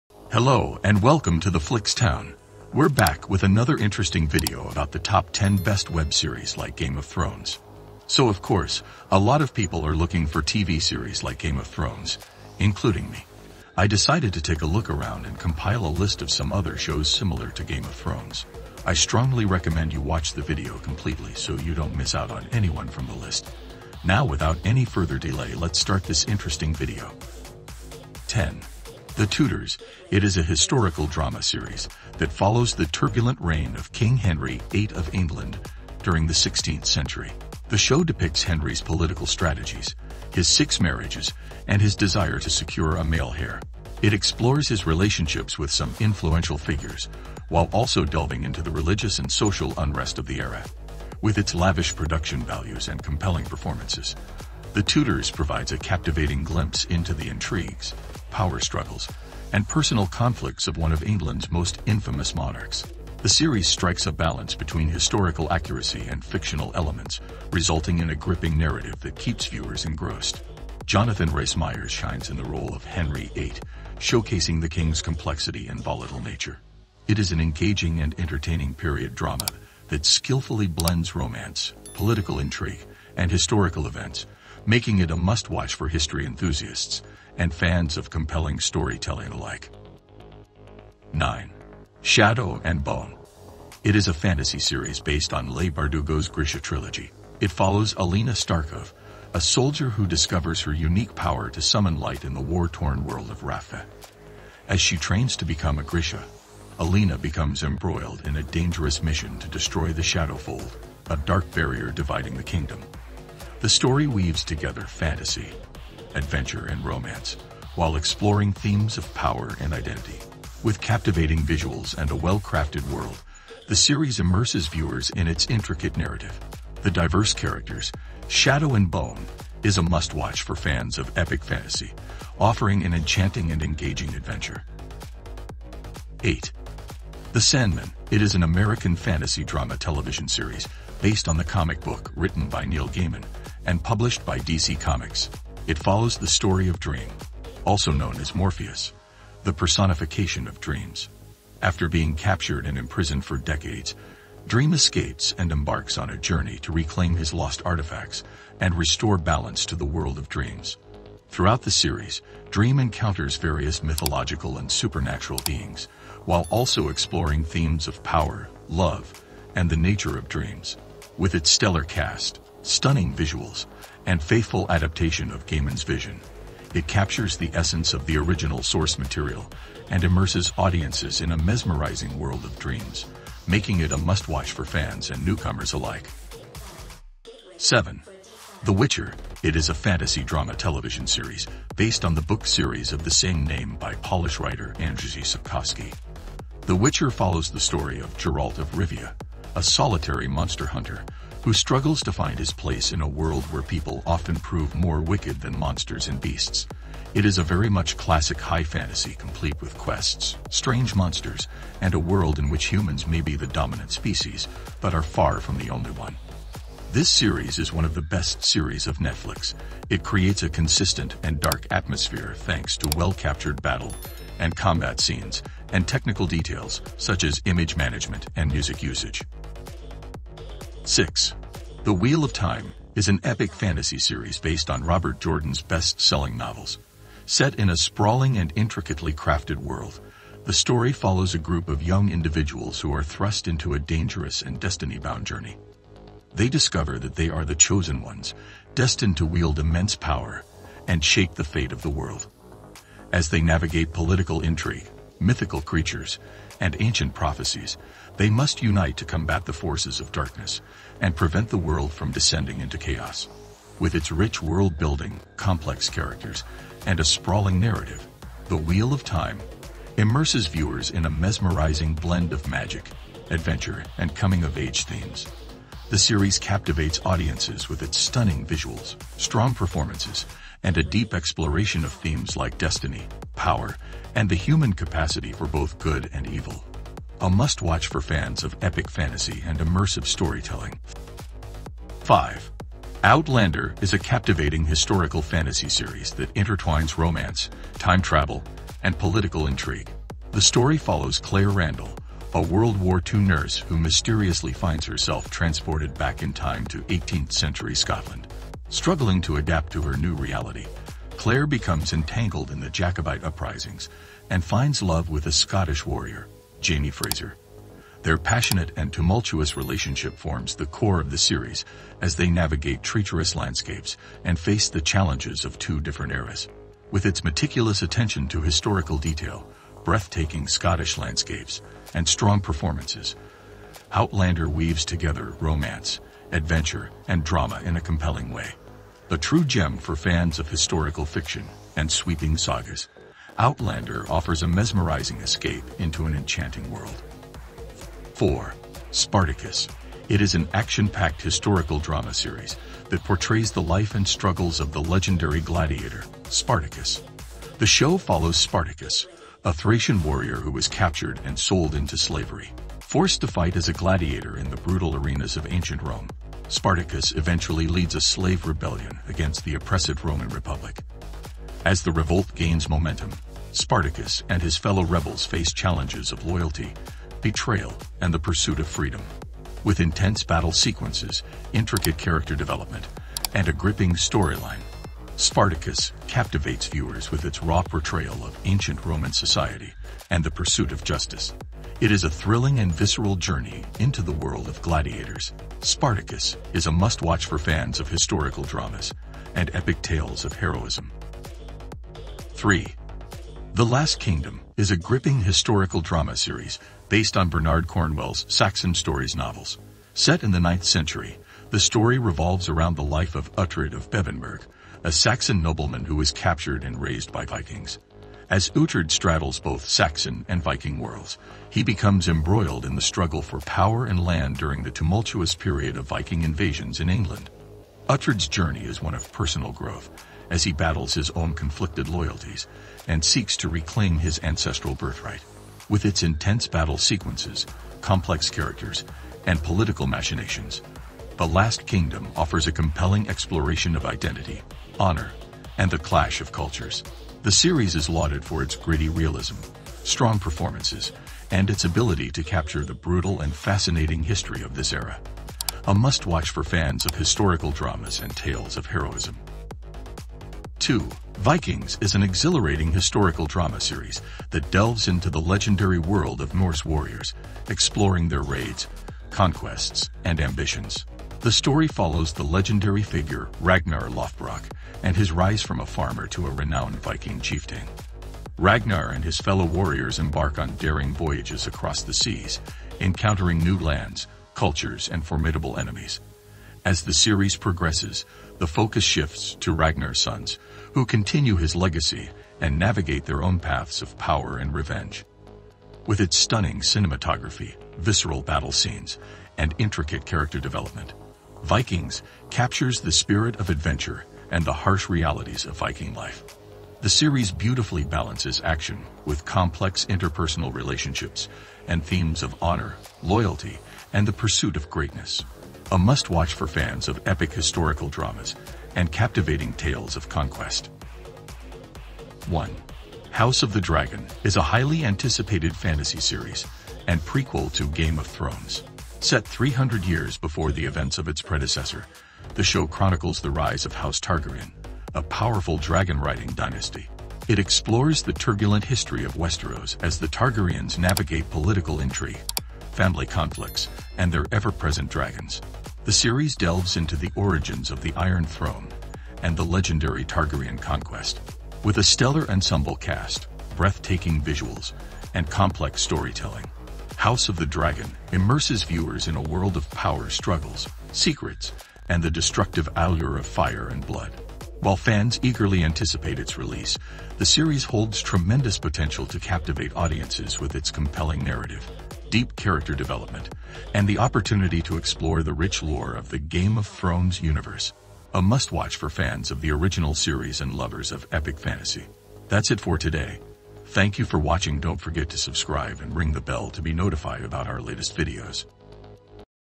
Top 10 Best TV Shows Like Game of Thrones | Similar Web Series Like Game of Thrones
In this video you will get the list of best shows as amazing as Game of thrones which are available on HBOmax, Netflix and amazon prime All these TV shows like game of thrones are fully loaded with, action packed scenes, fantasy, drama, twist, suspense and Wars. watch the full video for the Similar Web series to Game of Thrones.
Game of Thrones first premiered over 12 years ago on April 17, 2011, and whether you loved it or hated it, one cannot ignore the fact that the show became a worldwide phenomenon. The on-point casting, jaw-dropping visuals, intricately written internal political conflicts, and gruesome battle scenes were some of the key ingredients that made the show a decade-defining pop culture icon.

We have tried our best for making our video error-free. This is for educational and entertainment purposes only.